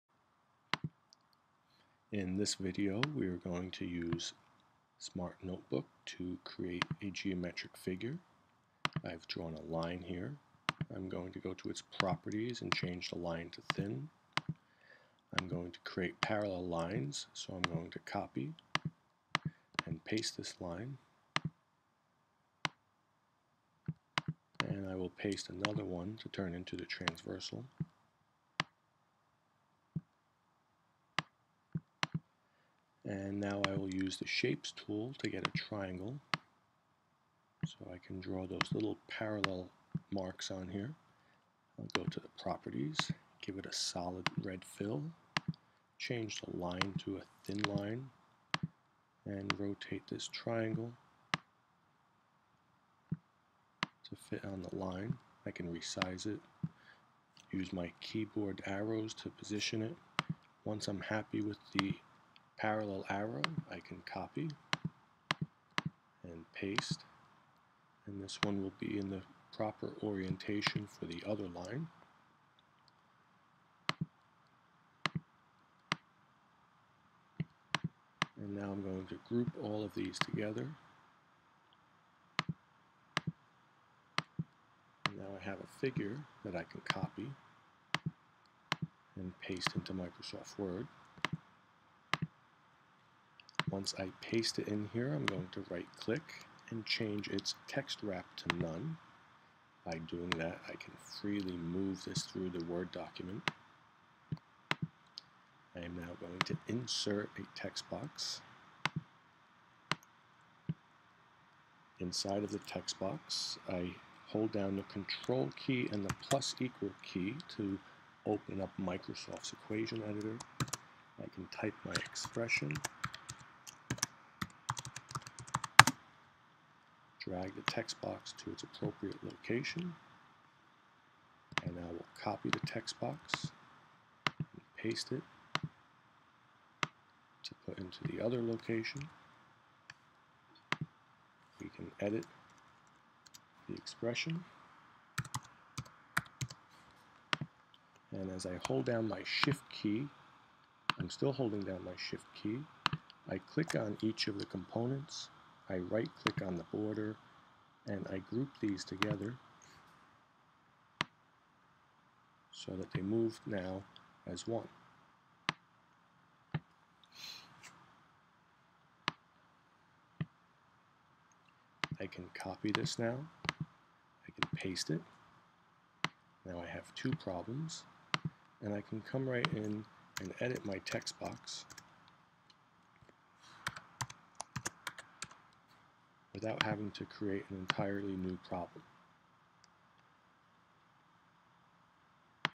Notebook & MS Word
Use Smart Notebook to generate graphics which can be copied and pasted into a Word document to create a worksheet or assessment.